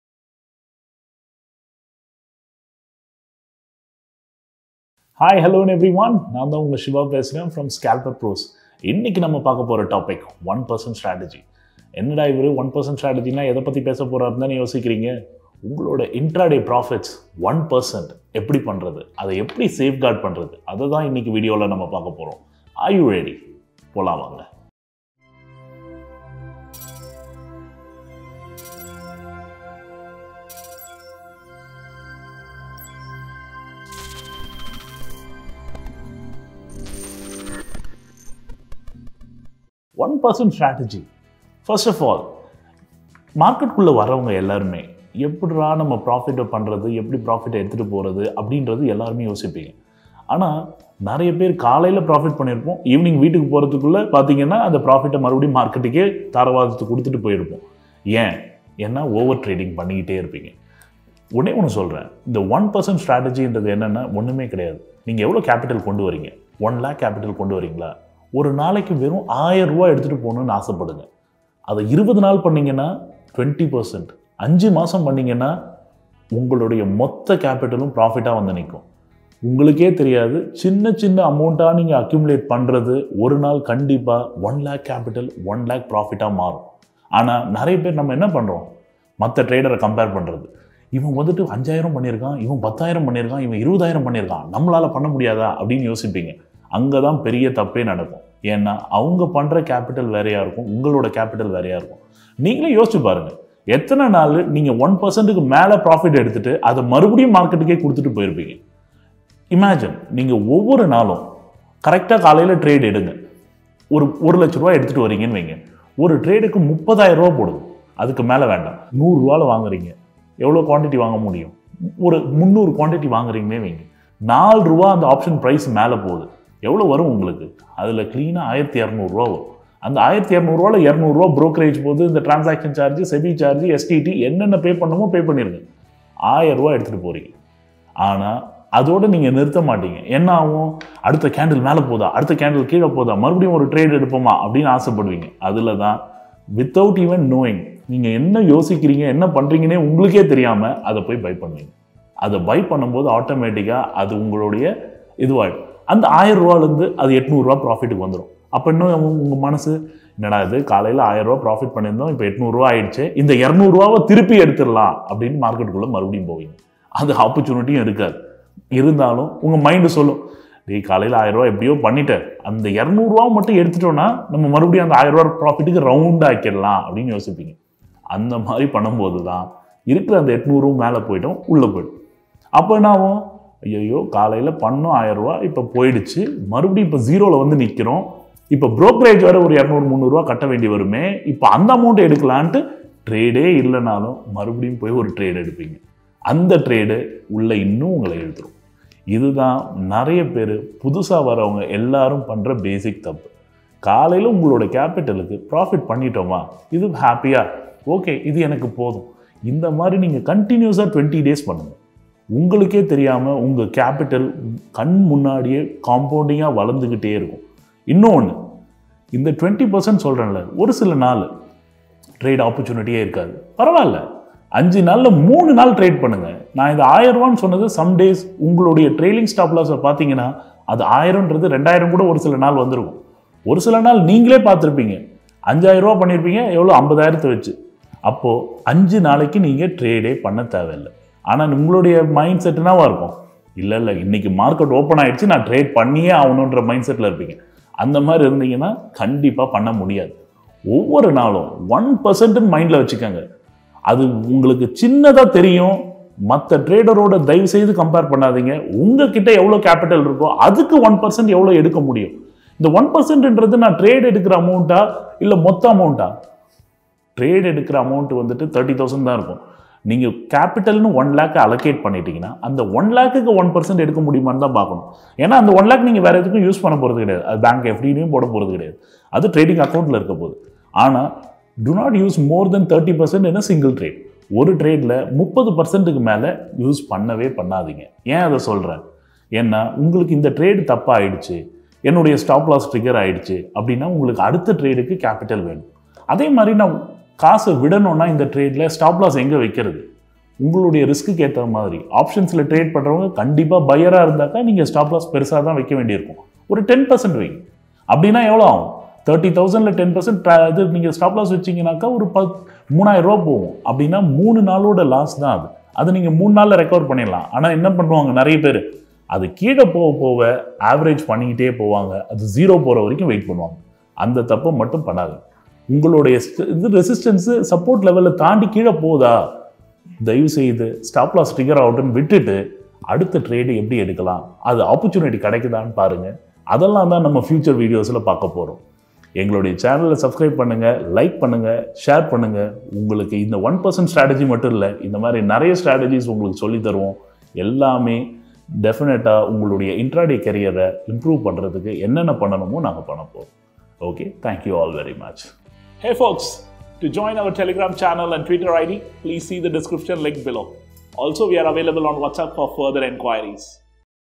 1% Strategy Explained | தமிழ் | Sivakumar Jayachandran | Scalper Pros
In this video an expert of stock market Mr. Sivakumar Jayachandran
explains 1% Strategy.